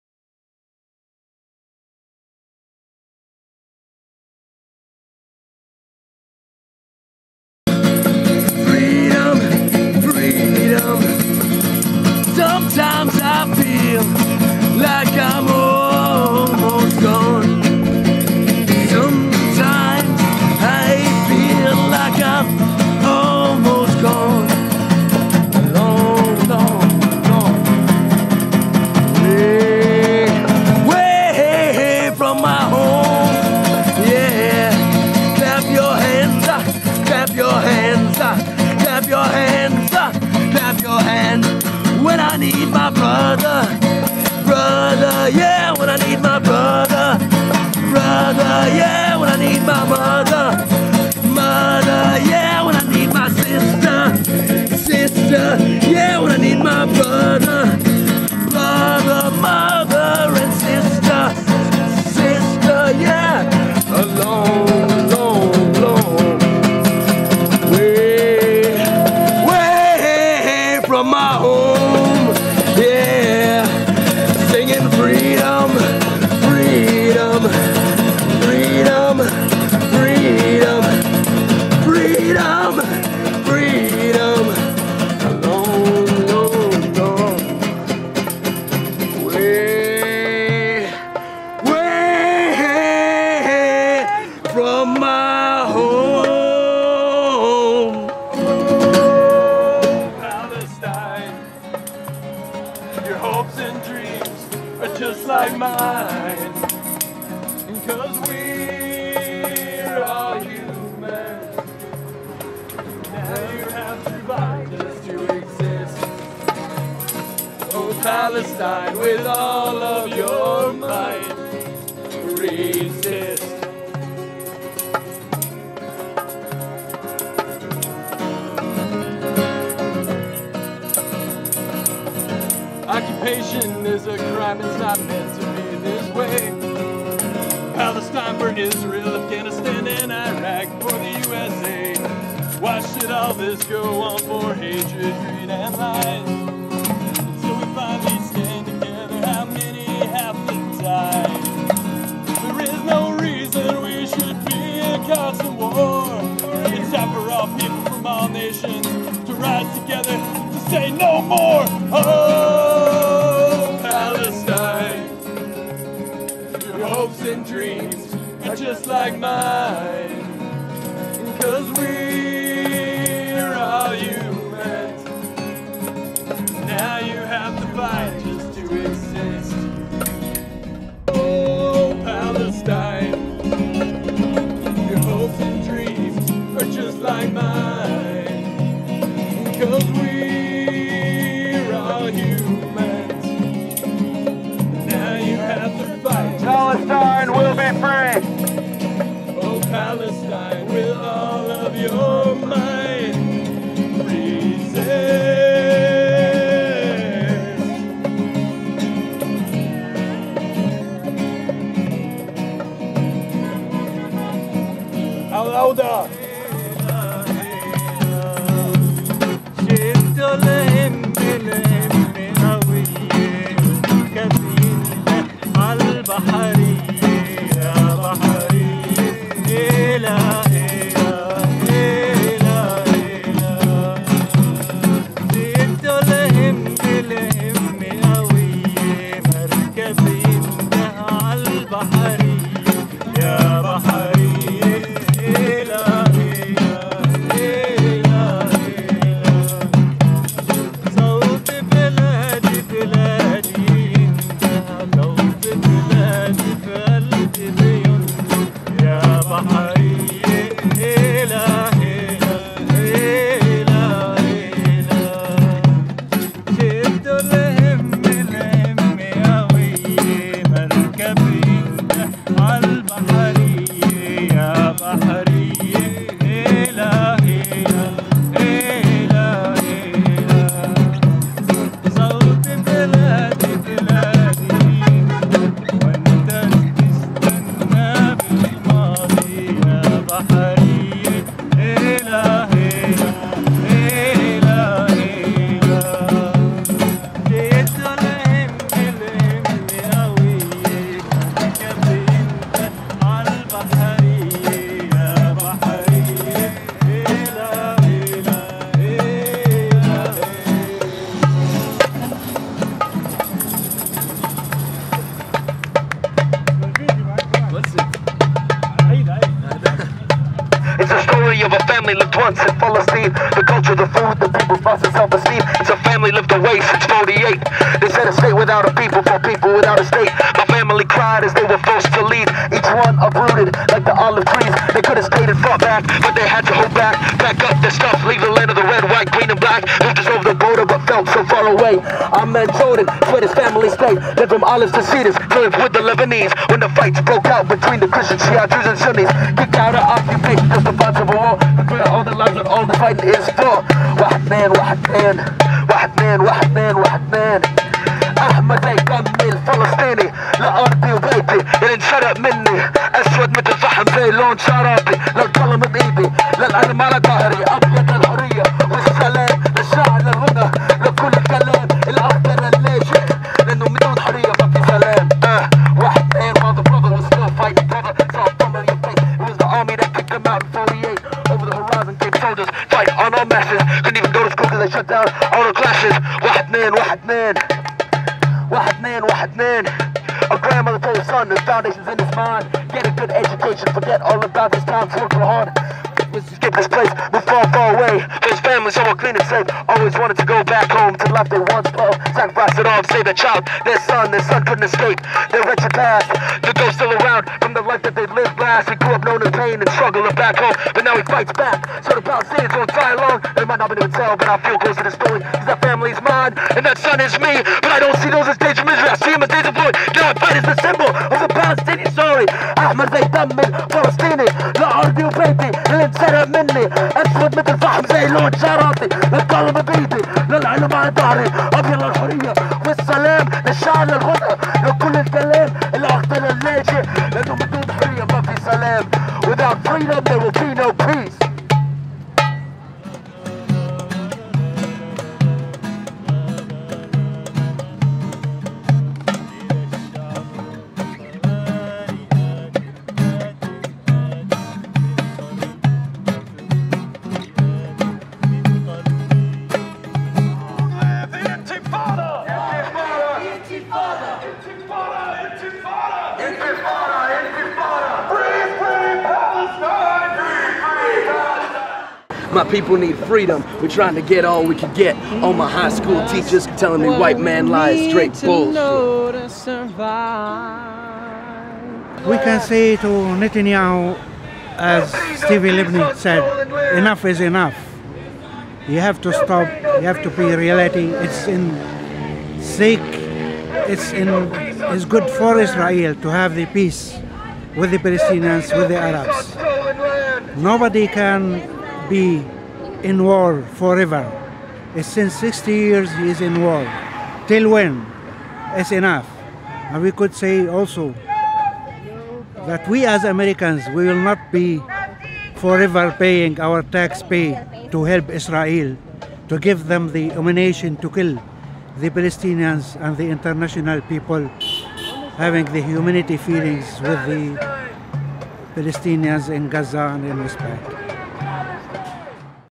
Solidarity Music - Emergency Protest Against Israeli Attacks on Gaza Freedom Flotilla
This Musical Performance and Video is Dedicated to the Courageous People in the Freedom Flotilla for Gaza and to the Palestinian People. We stand with you. We mourn with your family and friends today, on this BLOODY Memorial Day Massacre Monday. Here, in Austin Texas in front of Whole Foods, at Sixth Street and Lamar, we spent the afternoon of the Memorial Day Massacre in protest of Israel's murderous massacre of 19 Peace Volunteers who were killed in an illegal raid on May 31, 2010. The terrorist state of Israel boarded the ships in INTERNATIONAL waters and committed unspeakable acts of violence against humanitarian workers and activists. We stand in solidarity with the Freedom Flotilla, all of Palestine and against these acts of violence. This is a ZGraphix production. Produced by Jeff Zavala